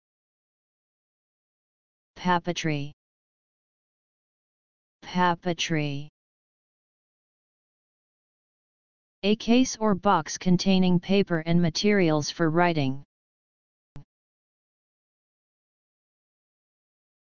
Papeterie Meaning
A case or box containing paper and materials for writing.

In this video, you will learn "Papeterie" Meaning and pronunciation in English.

Wordy Affair